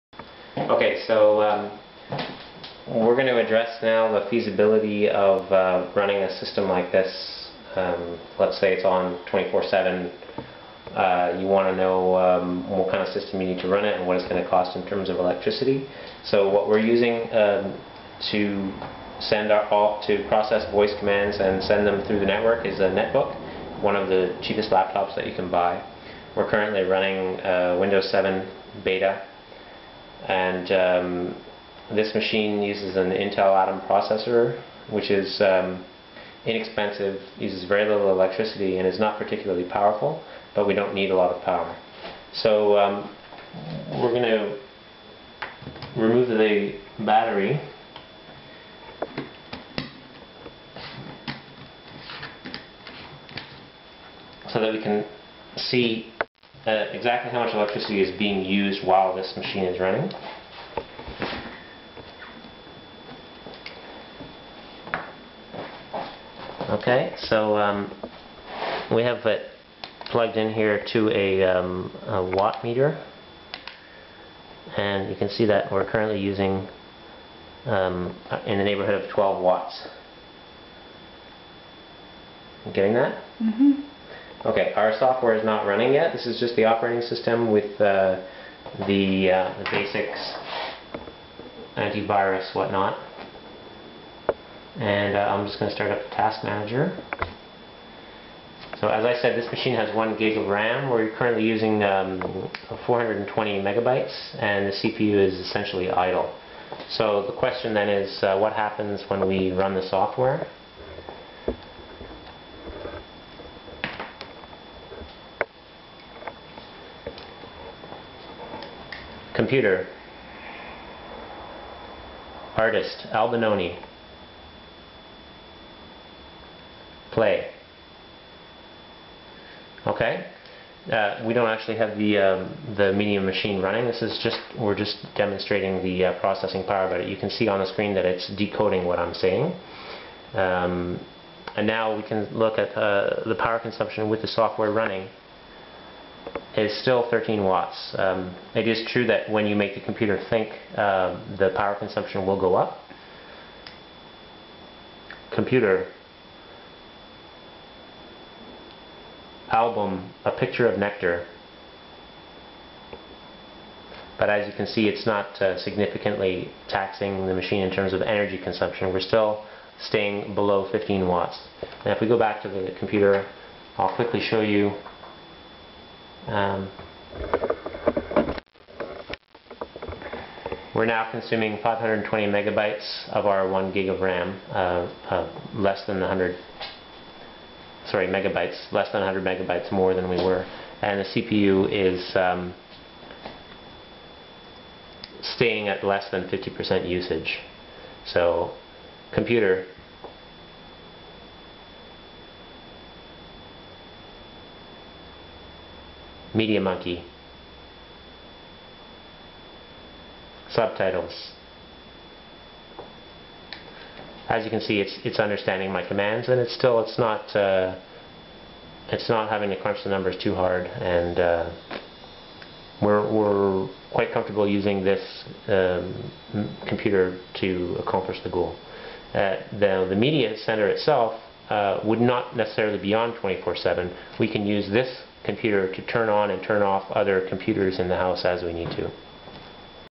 Vox Commando 03 - System and Electricity
Vox Commando - htpc voice recognition: what is the energy consumption and processing power required.  Speech recognition running on Acer Aspire One netbook (d150)
powerconsumption remains below 15 watts